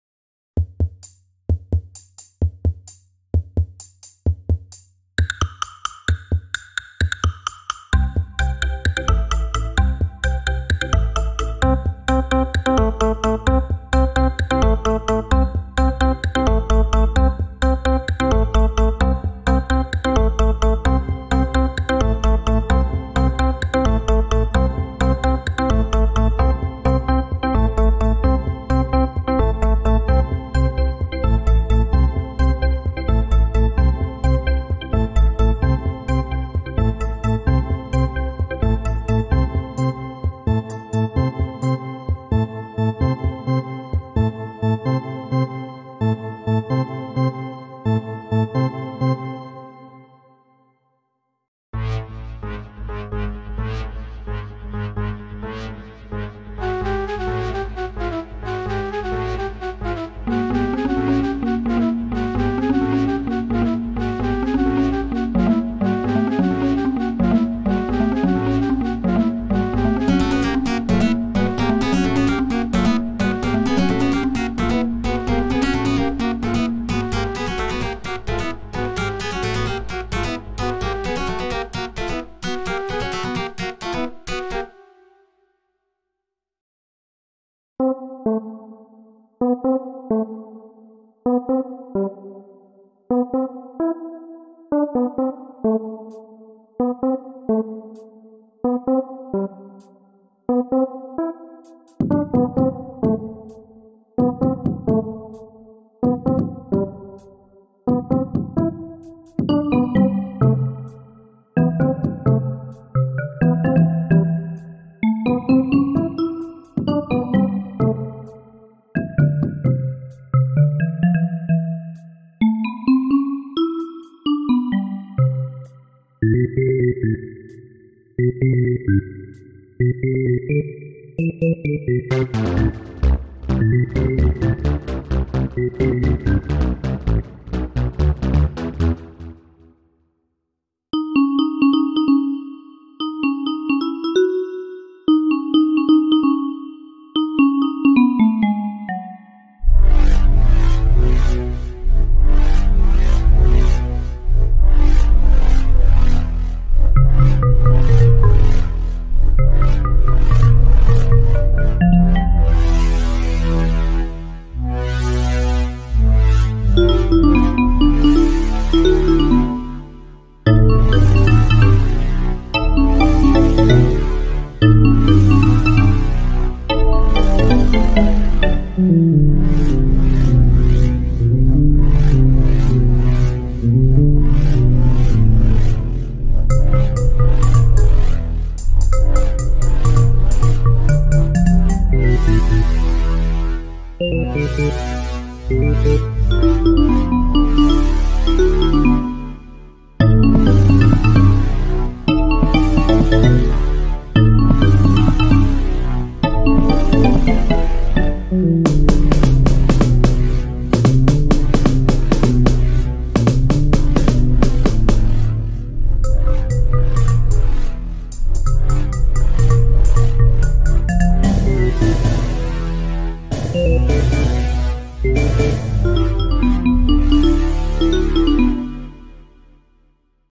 My Music: WIP #14 - #17
Some more songs I made in FL Studio. The first one is a percussion loop with more instruments added each time around, and the second one is just a short simple loop with a synthesizer, flute, marimba and guitar. The last two I made a 30 second loop that played 3 times and added more each time around, rather than a shorter loop played more times around. This culminated in WIP #18, which is a longer 3 minute song, albeit still with a lot of repetition a la loops.